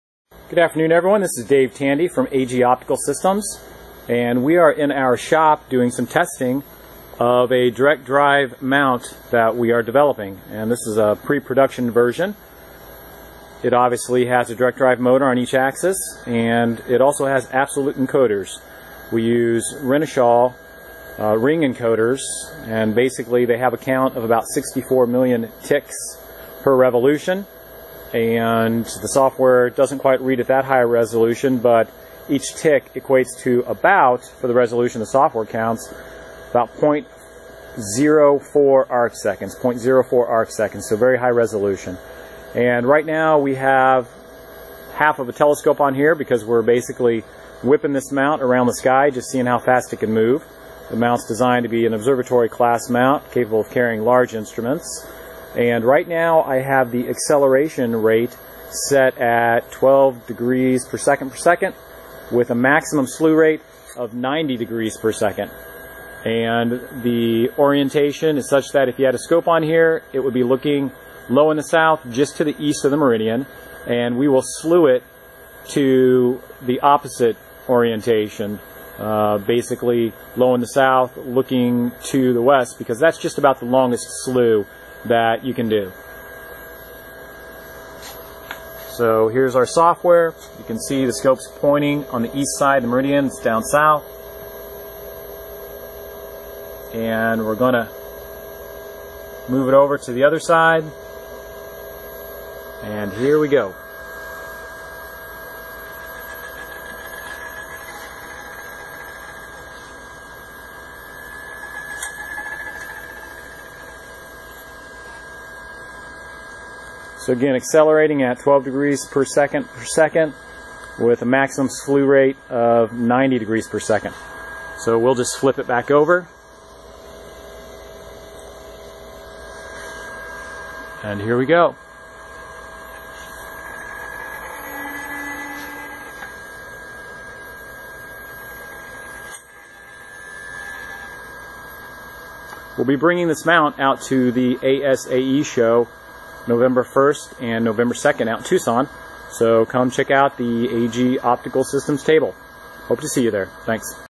AG Optical DD150 video
Video of the AG Optical Systems DD150 direct drive German equatorial mount slewing at a high rate.  This a preproduction version of the mount.